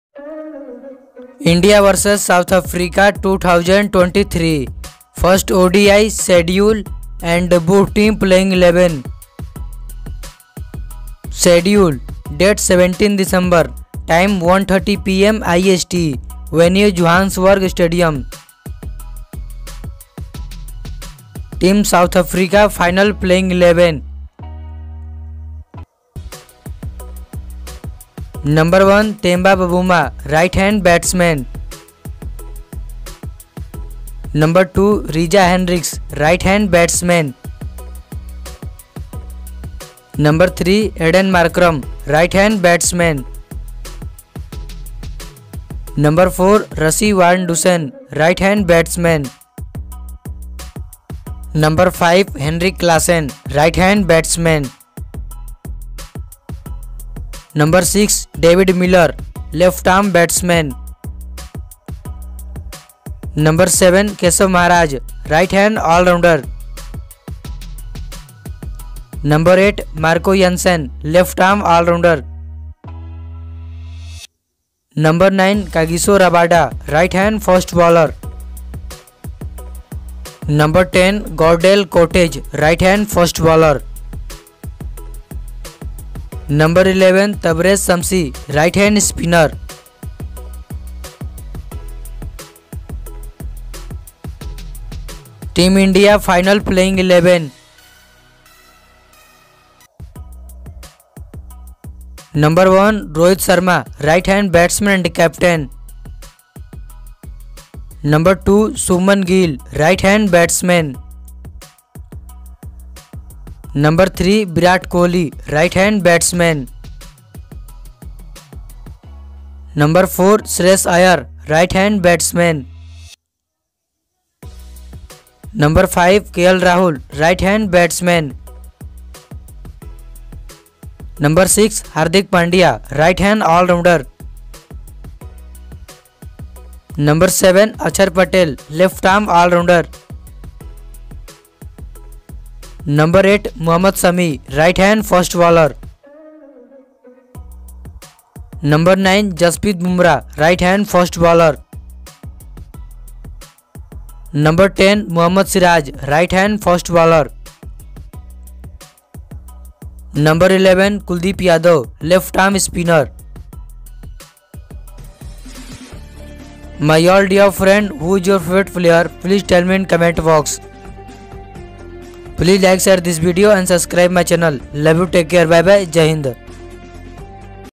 India vs South Africa 1st ODI Match 2023 | Ind vs Sa 1st ODI Playing 11
Match Details.

Venue - Johannesburg

India Playing 11 vs South Africa

Rohit Sharma, Shubman Gill, Virat Kohli, Sheryas Iyer, Kl Rahul, Hardik Pandya, Ravindra Jadeja, Kuldeep Yadav, Jasprit Bumrah, Mohammed Shami, Mohammad Siraj

South Africa Playing 11 vs India

Reeza Henrikes, Temba Bavuma, Rassi Van Der Dussen, Aidan Markram, Henri Classen, Devid Miller, Marco Jansen, Keshav Maharaj, Kagiso Rabada, Gerald Coetzee, Tabraiz Shamsi